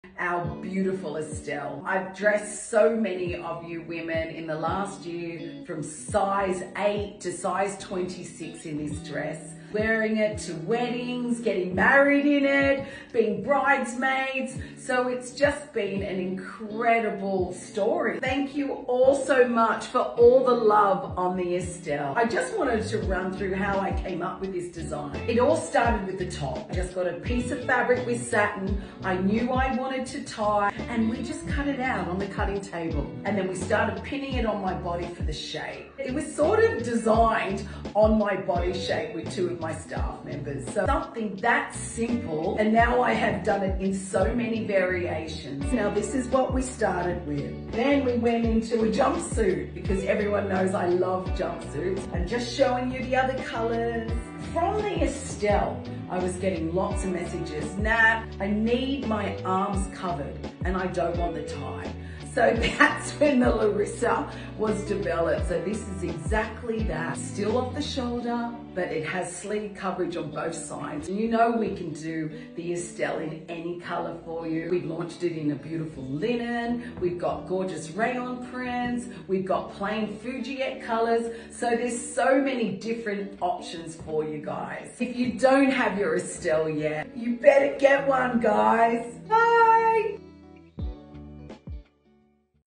The Estelle Design 🩷
Feeling incredibly grateful for the success of my Estelle design! 💖 Available as a top, dress, & jumpsuit, it looks absolutely stunning on all shapes and sizes. Thank you so much all for your love and support! 💕 Natasha x